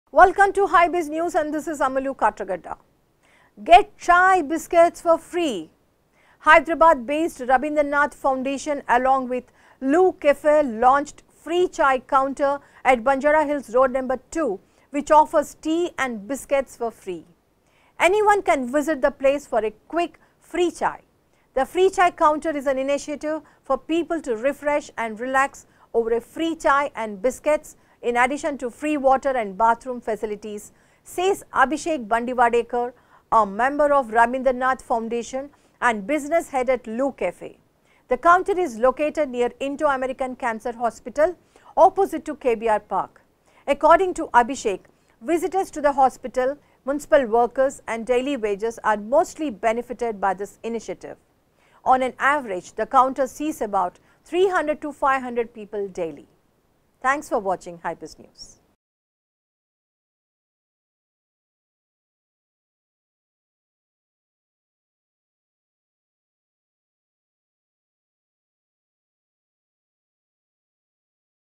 Get chai, biscuits for free | Hybiz tv
Hyderabad-based Rabindernath Foundation along with LooCafe launched ‘FreeChaiCounter’ at Banjara Hills Road No 2 which offers tea and biscuits for free. Anyone can visit the place for a quick, free chai. “The Free Chai Counter is an initiative for people to refresh and relax over a free chai and biscuits, in addition to free water and bathroom facilities,” says Abhishek Bandiwadekar, a member of Rabindernath Foundation and Business head at Loo Cafe. The counter is located near Indo-American Cancer Hospital, opposite KBR Park. According to Abhishek, visitors to the hospital, municipal workers, and daily wagers are mostly benefitted from this initiative. On average, the counter sees about 300-500 people daily.